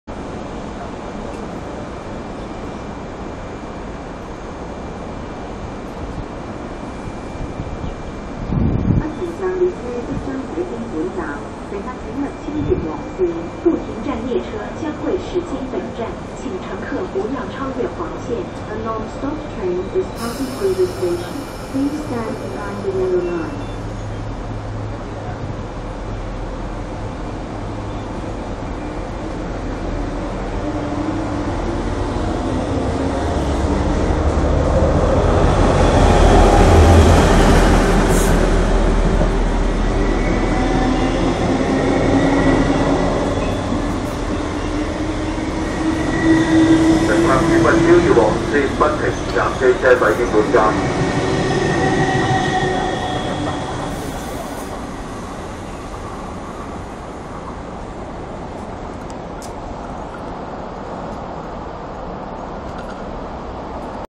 {ERL} Two different Diesel Locomotive ( ER20 + G26CU ) towards Depot
*The freight services of MTR East Rail Line had end since 26th June,2010

Locmotive : 8004 (ER20)  , MTR62 (EMD G26CU)
Freight train no. : - - - - - -
Route : From Lo Wu Freight Yard to Depot

Place : MTR ShaTin Station
Video by : ERL62train
Camera : Samsung Galaxy Ace
Record Date : 08/10/2011